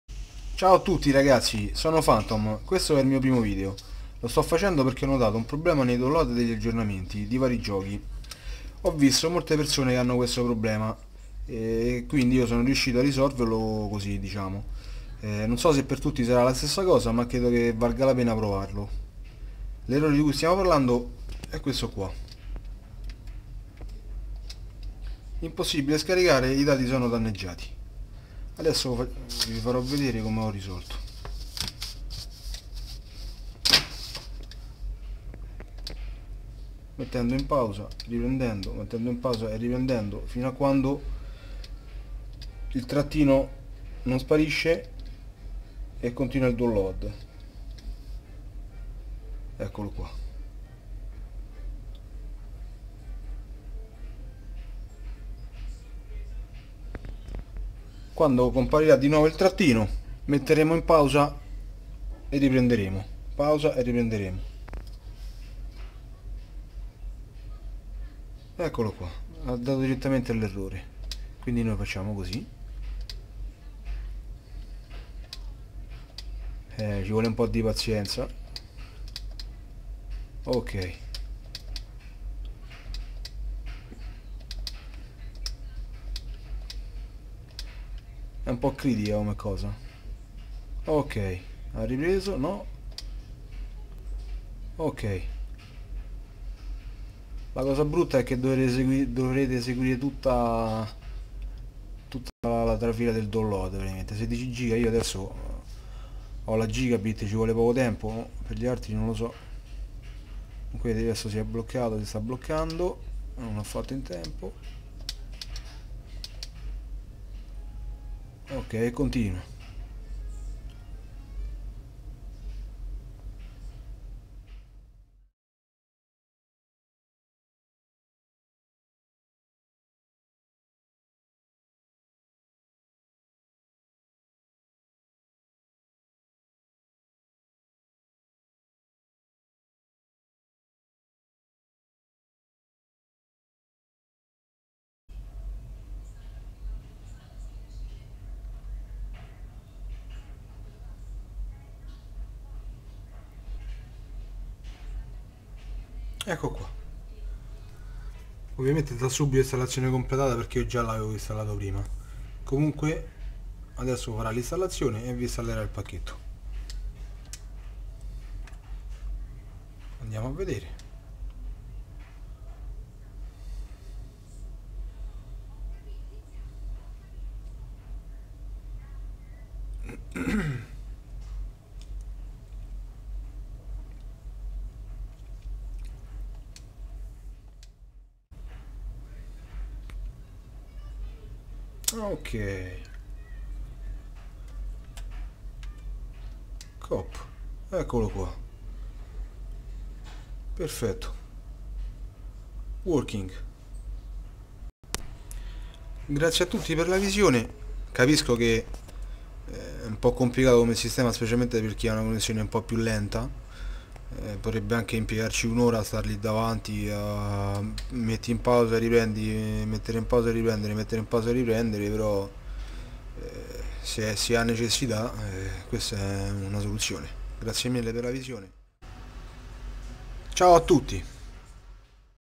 come risolvere il problema/errore CE-36244-9 PS4 dati danneggiati
Ciao ragazzi, in questo video propongo una soluzione al problema dei download degli aggiornamenti di qualche gioco della ps4, non so l'errore da cosa sia dovuto e non so se questa soluzione potrà andare bene per tutti, ma l'ho testata ed ho visto che funziona. Per questo voglio condividerla con voi, Grazie